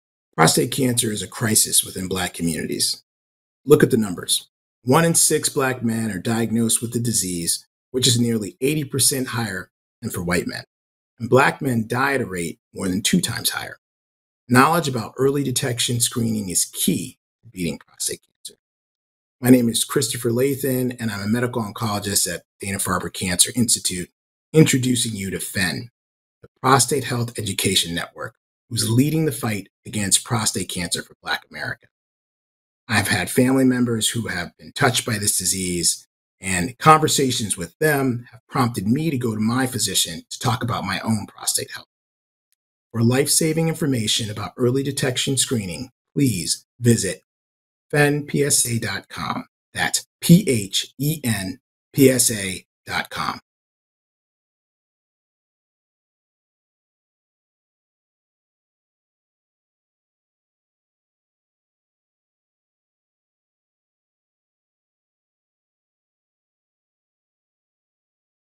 Friends of Dr. Chris Lathan encouraged him to see doctor about his own prostate health.
Dr. Christopher Lathan, medical oncologist at Dana-Farber Cancer Institute in Boston shares his experience with family and friends encouraging him to get his own prostate health.  Visit phenpsa.com for more information about early detection prostate cancer screening.